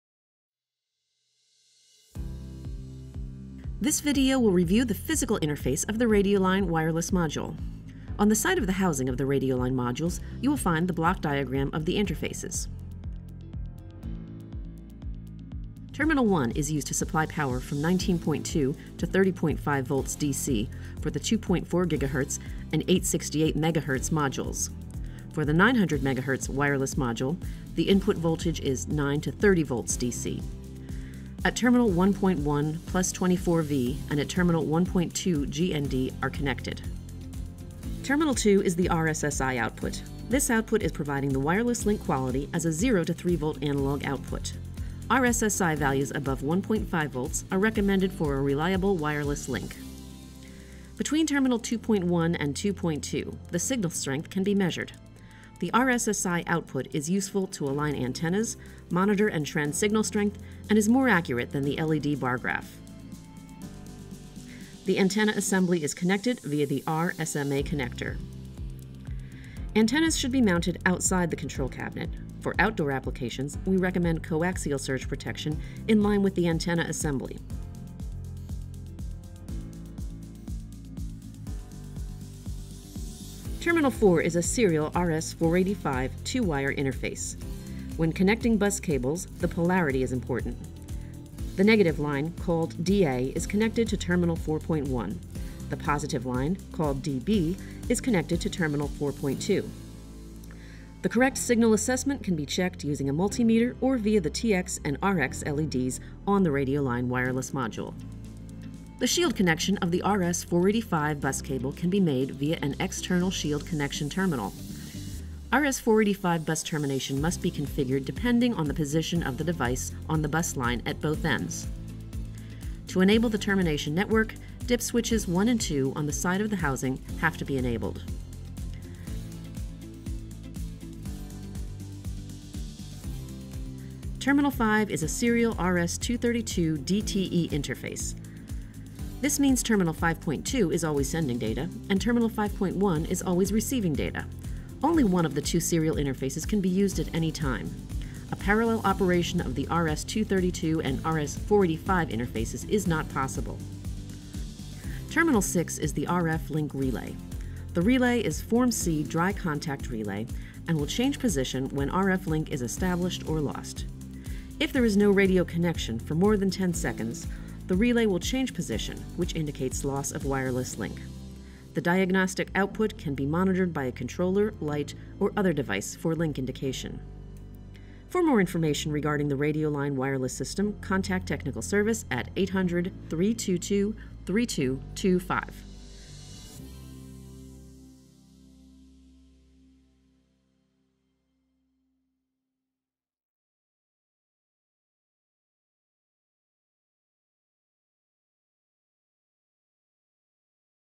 Radioline – Wireless module Interfaces
►This video explains all interfaces of the Radioline wireless module. Additionally many backgrounds and content from the user manual are discussed.